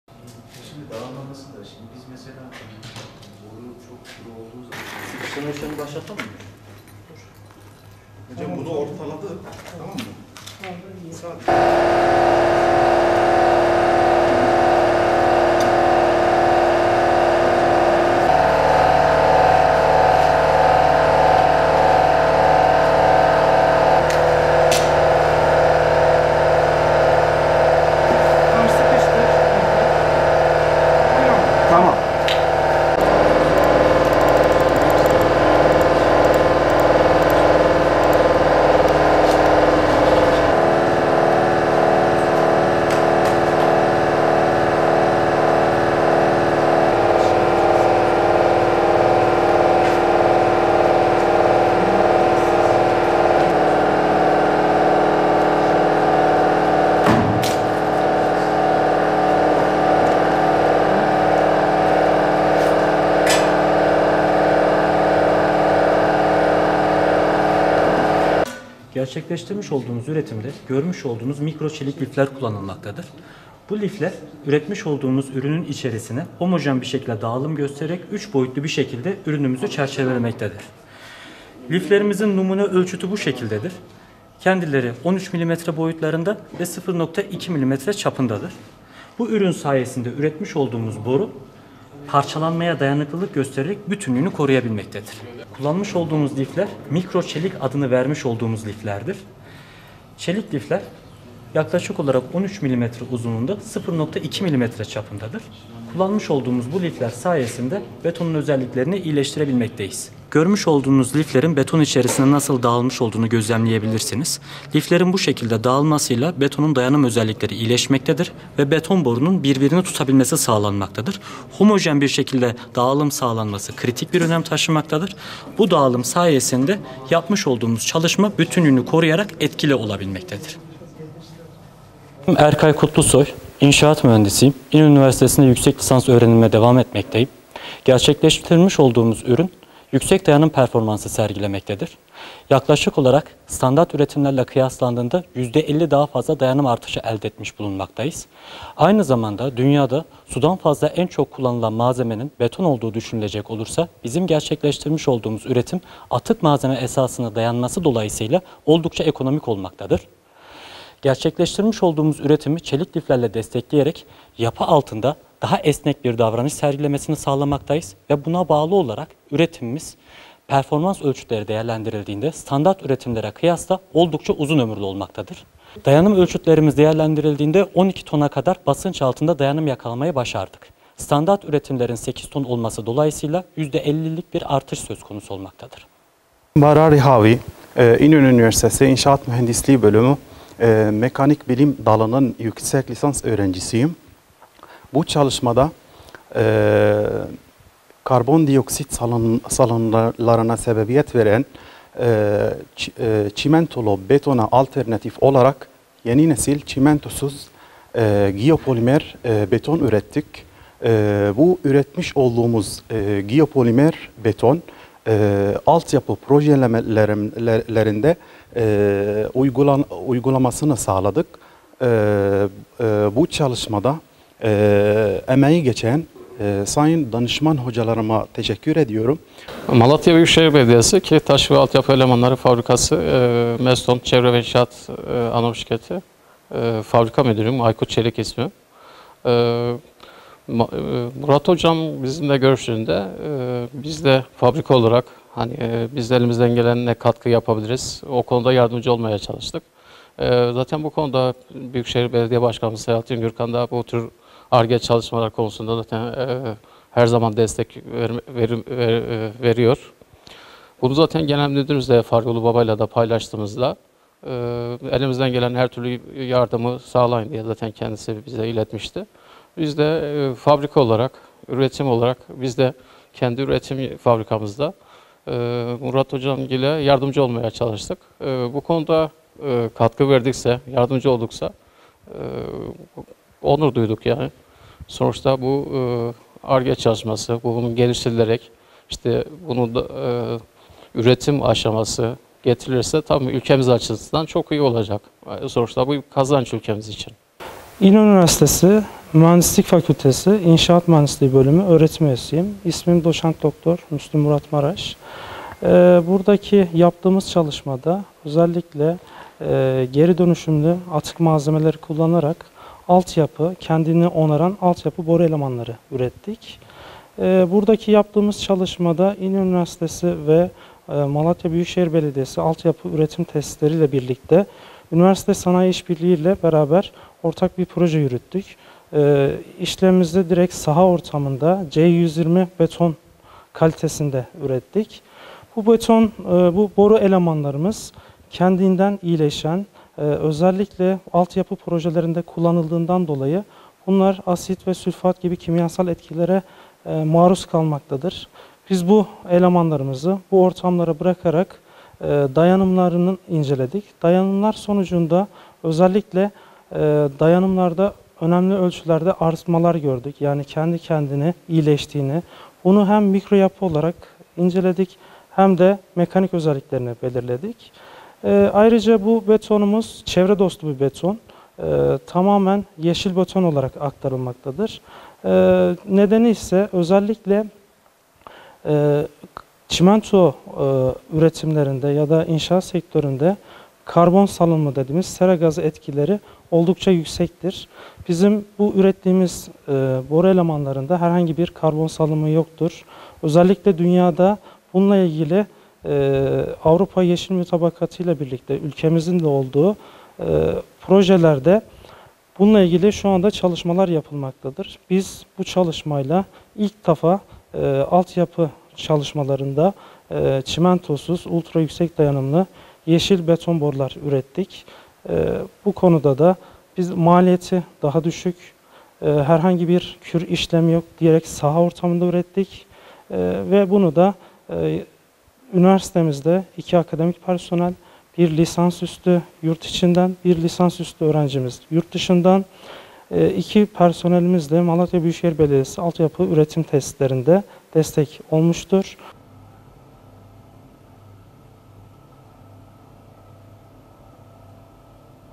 Malatya'da Altyapı Teknolojisinde yeni bir çığır Daha
Malatya’da İnönü Üniversitesinde görevli Doç. Dr. Müslüm Murat Maraş'ın öncülüğünde 5 kişilik bir ekip, atık malzemeler kullanılarak 12 ton basınca dayanıklı beton boru üretti.